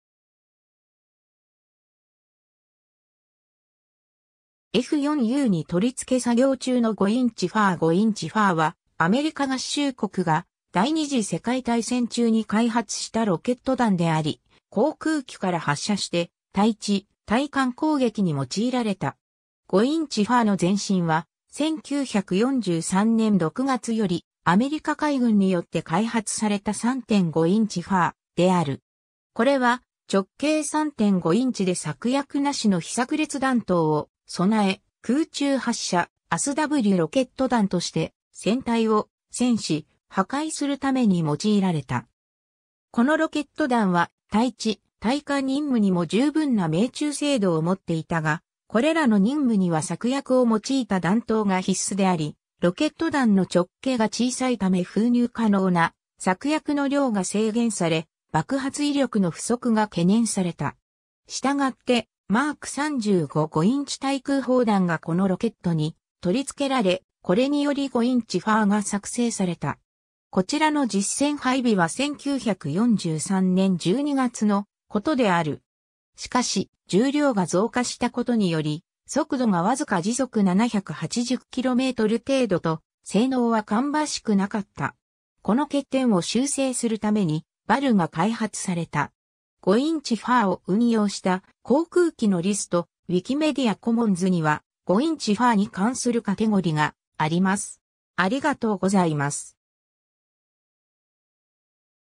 5インチ FFAR
F4Uに取り付け作業中の5インチ FFAR 5インチ FFAR（5-inch Forward Firing Aircraft Rocket、5インチ前方発射航空ロケット弾）は、アメリカ合衆国が第二次世界大戦中に開発したロケット弾であり、航空機から発射して対地・対艦攻撃に用いられた。
 5インチ FFARの前身は、1943年6月よりアメリカ海軍によって開発された3.5インチ FFARである。
これは直径3.5インチ（約8.9cm）で炸薬無しの非炸裂弾頭を備え、空中発射ASW（対潜水艦戦）ロケット弾として船体を穿刺・破壊するために用いられた。
 このロケット弾は対地・対艦任務にも十分な命中精度を持っていたが、これらの任務には炸薬を用いた弾頭が必須であり、ロケット弾の直径が小さいため封入可能な炸薬の量が制限され爆発威力の不足が懸念された。
したがってMk35 5インチ対空砲弾がこのロケットに取り付けられ、これにより5インチ FFARが作成された。
こちらの実戦配備は1943年12月のことである。
しかし、重量が増加したことにより速度がわずか時速780km(秒速216.6m)程度と性能は芳しくなかった。
この欠点を修正するためにHVARが開発された。
 5インチ FFARを運用した航空機のリスト： ウィキメディア・コモンズには、5インチ FFARに関するカテゴリがあります。